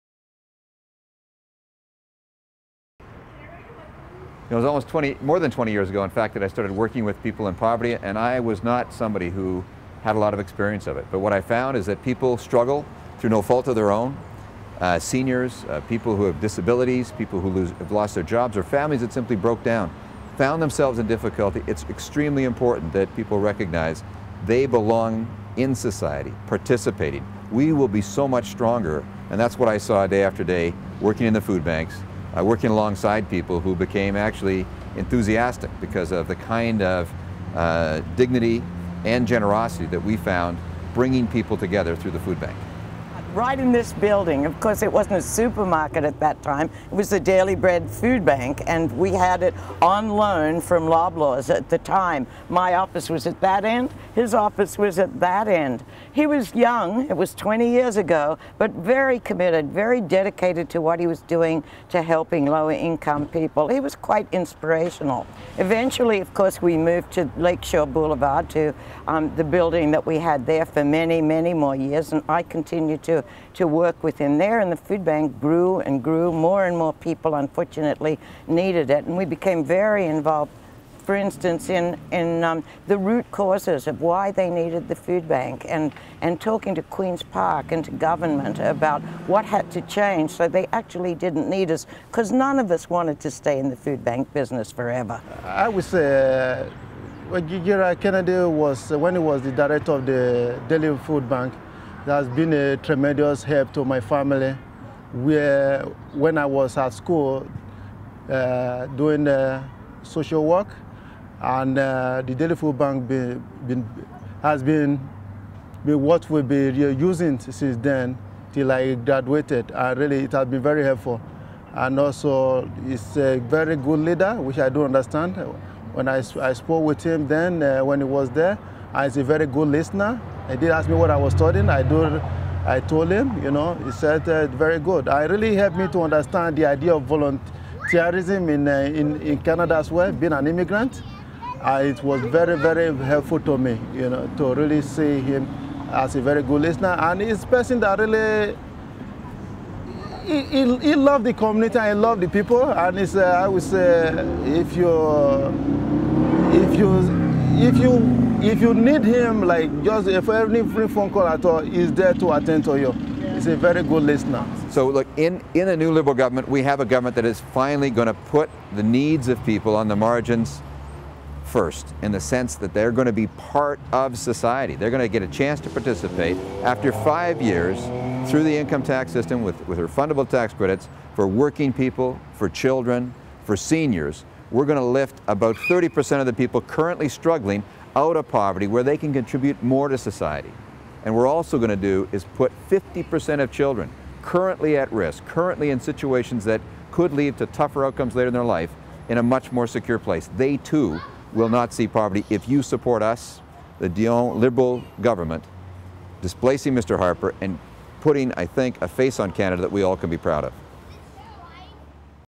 Gerard Kennedy on your FOOD BANK (Liberal, Parkdale-High Park)
On TUESDAY, OCTOBER 14th -- Vote GERARD KENNEDY and the new Liberal Government to represent Parkdale-High Park!